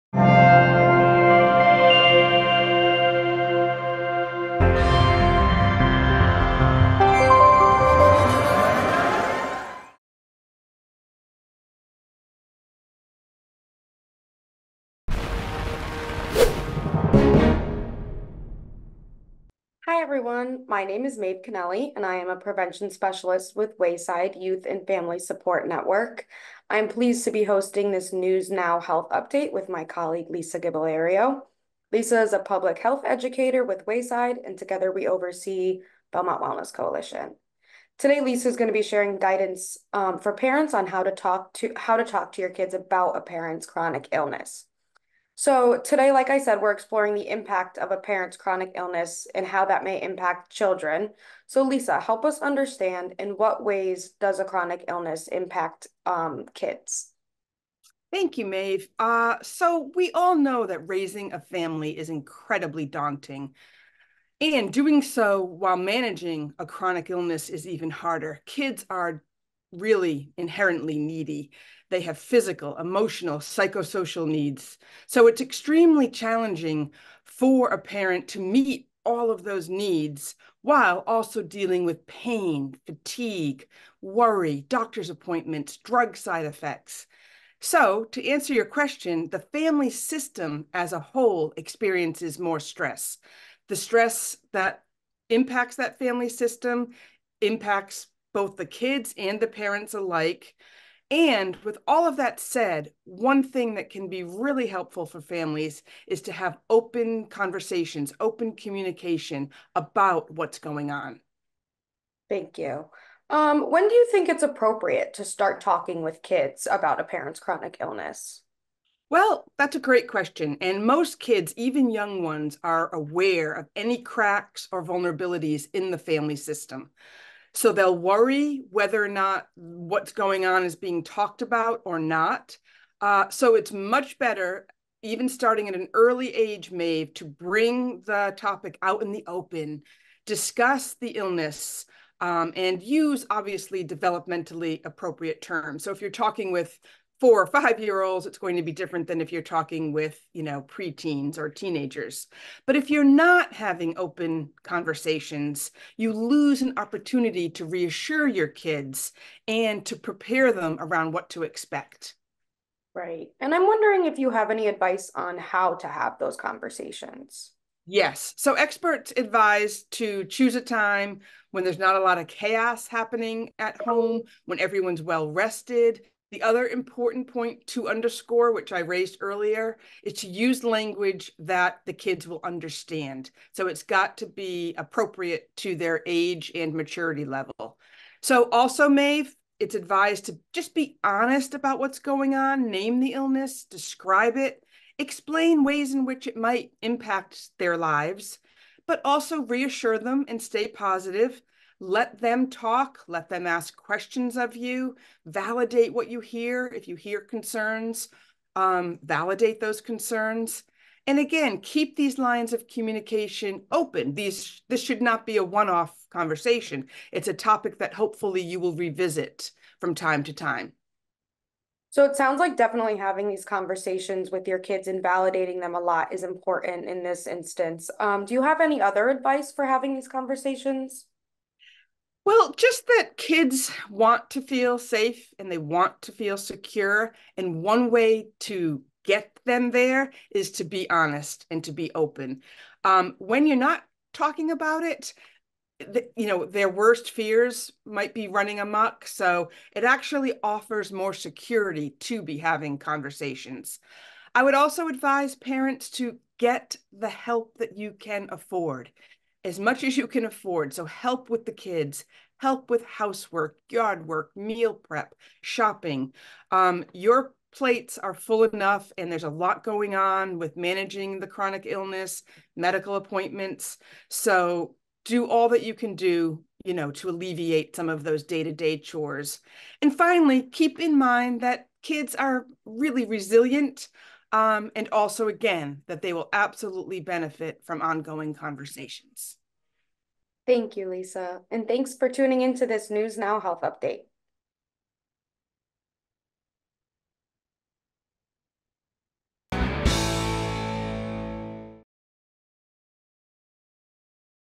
News Now: Health update with Lisa Gibalerio
Lisa Gibalero, MPH, PUblic Health Educator at Wayside Youth and Family Support Network, offers guidance on how to talk with kids when one parent has a chronic illness.